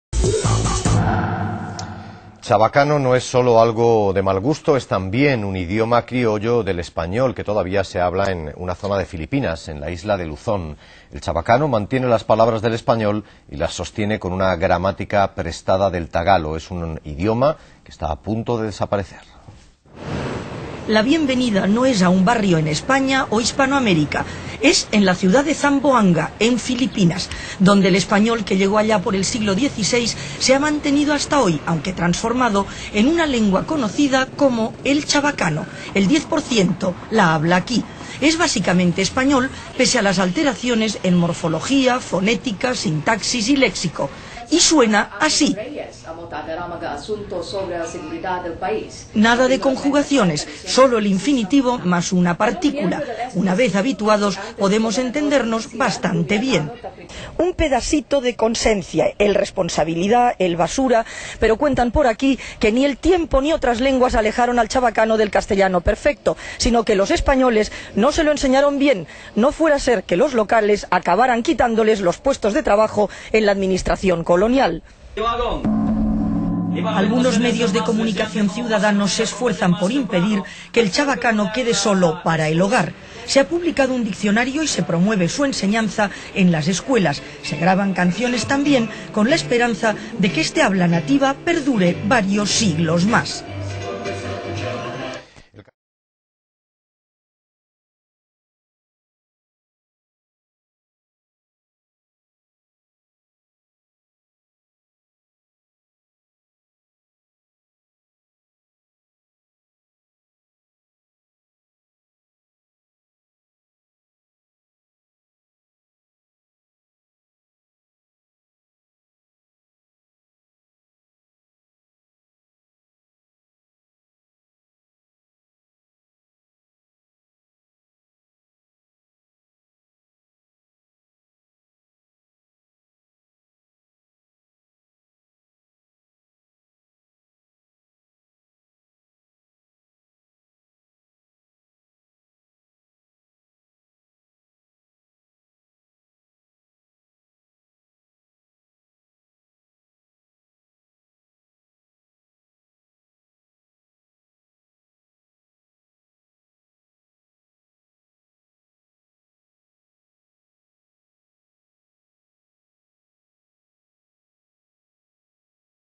TVE news report on Chavacano
I've been playing around with Windows Movie Maker and decided to make a video with subtitles.

I chose a 2004 news report about the Chavacano language from TVE, a TV station based in Spain.

Enjoy the video. I speak Spanish, but had some difficulty with translating certain phrases. But I think it's pretty much understandable.

Since I study Philippine languages and linguistics, I put some of my own commentary which is loosely based on a blog post I did on this back in 2004.

additional keywords: español languages filipinas ilonggo criollo pidgin